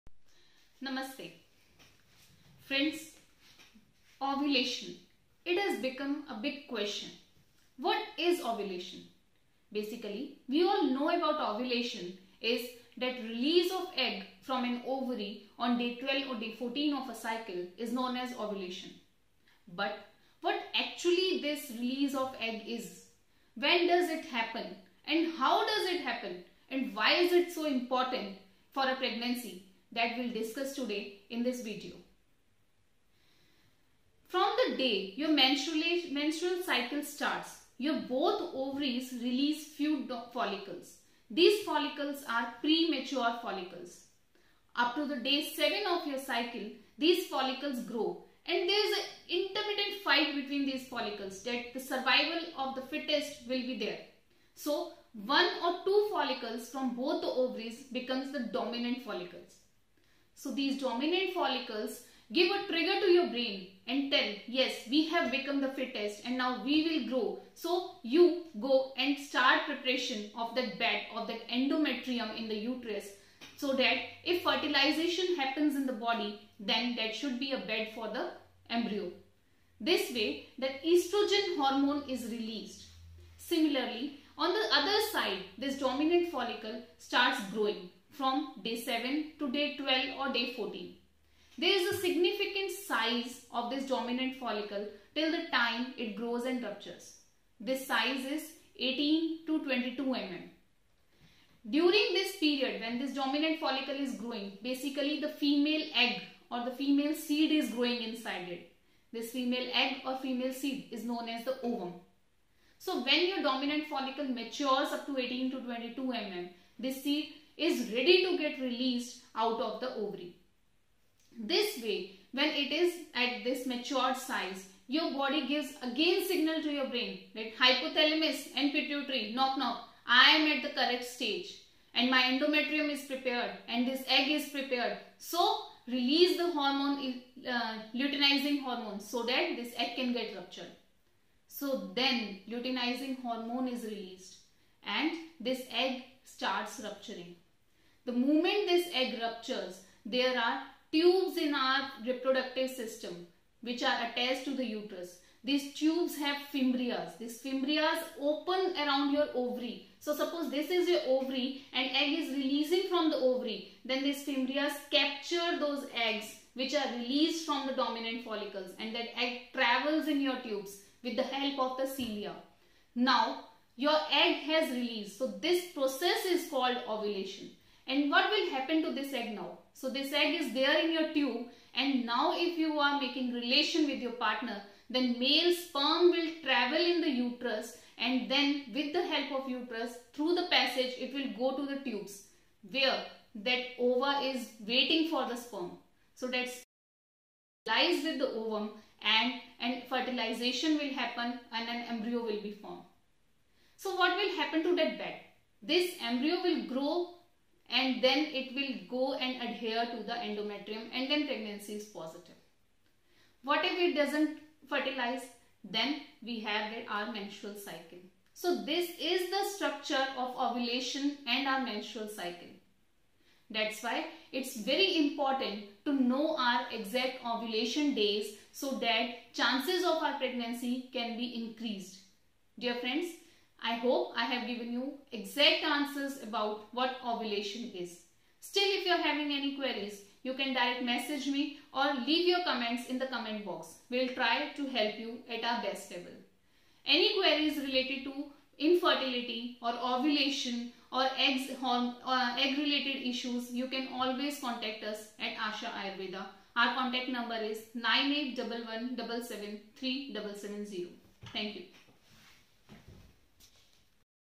What is Ovulation in English
Know when is the right time to become pregnant it is important to know about ovulation?

What is ovulation often the question of women who are not well aware of it? Today we will try our best to give all the information related to ovulation in this video. Such as when ovulation occurs, how to find ovulation, how to know ovulation, what is the time of ovulation, when does ovulation period come, what is ovulation cycle, how to get ovulation date, how long does ovulation last, ovulation How to track it with the help of a calculator, it will answer questions, etc. The well-known Fertility Expert Dr. Chanchal Sharma who has been serving his services continuously at the Asha Ayurveda Center for the last 10 years.

call Doctor any query related infertility :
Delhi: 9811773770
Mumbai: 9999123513